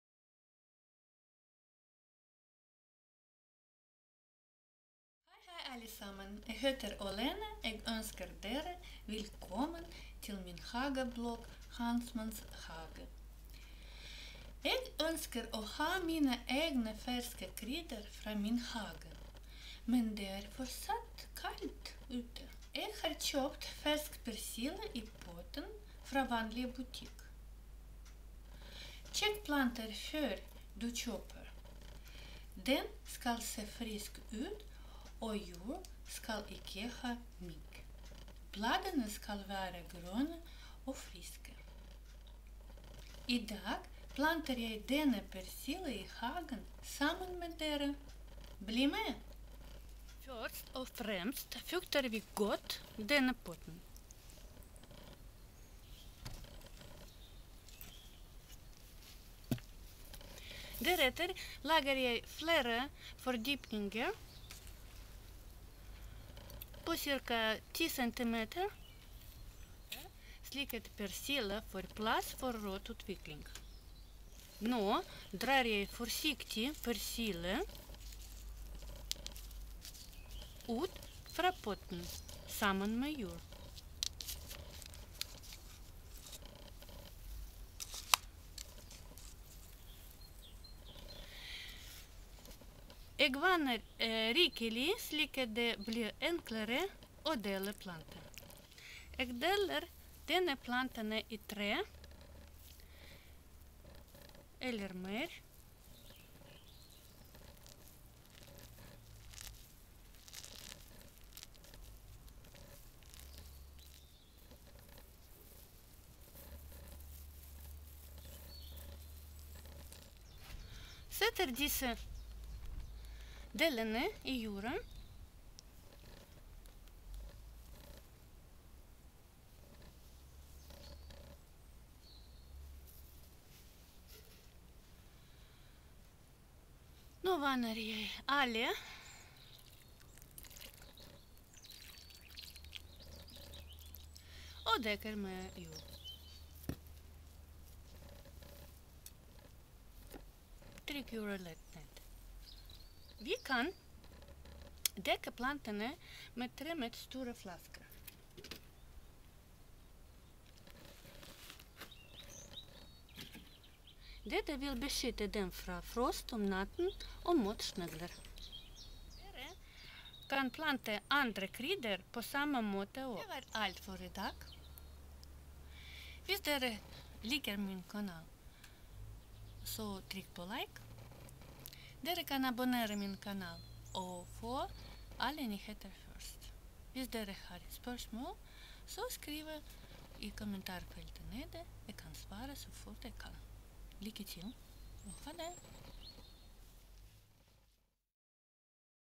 8. Hvordan planter vi persille fra potten
Det er så godt å ha ferske krydder fra sin hage. Vi kan plante krydder fra potten som kjøpes i vanlige butikken og da har vi frisk krydder fortere... Dere kan se i den video hvordan skal vi gjøre det...